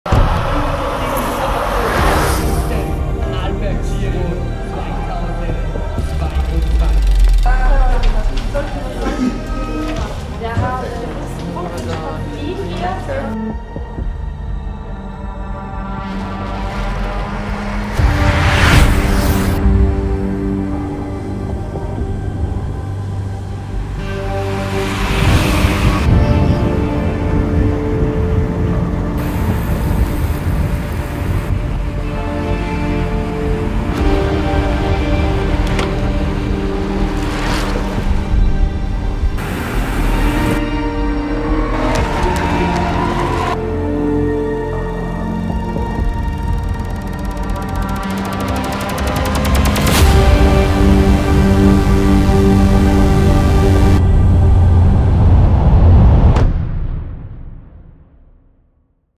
ARLBERG Giro 2023 - Trailer
ARLBERG Giro
am Sonntag, 30. Juli 2023

St. Anton – Arlbergpass – Bludenz – Montafon – Silvretta – Paznauntal – St. Anton
150 km, ca. 2.500 Höhenmeter im Anstieg